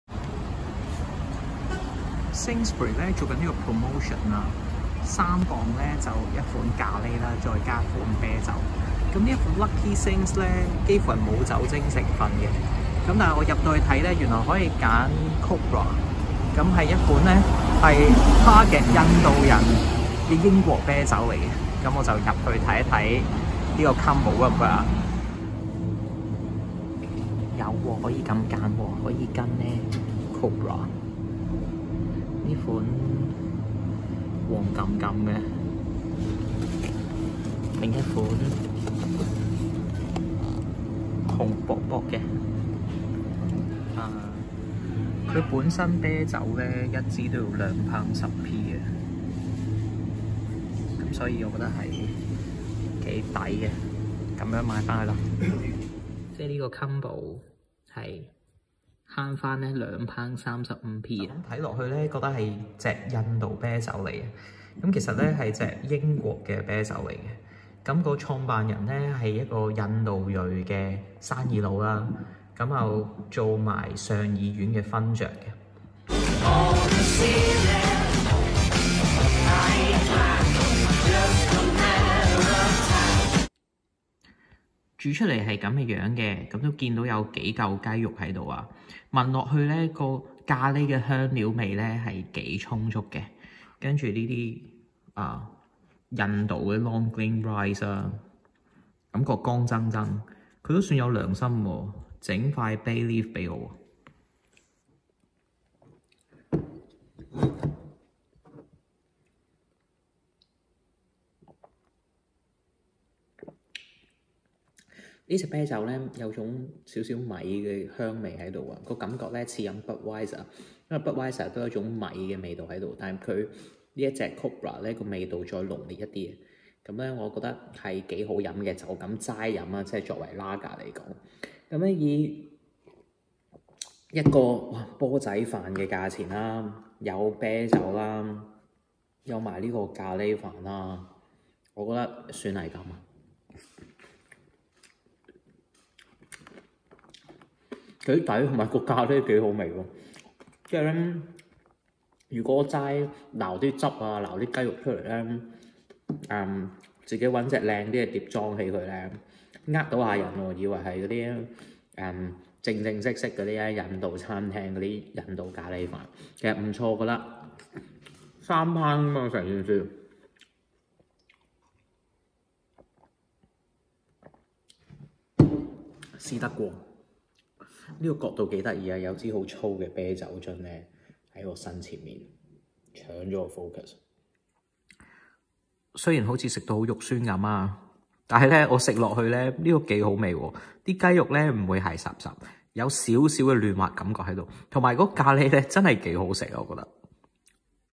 Junk with Beer #15 - Sainsbury‘s meal deal Cobra premium lager with curry rice
Junk: Chicken Tikka Masala with pilau rice
Beer: Cobra premium lager
A combo of £3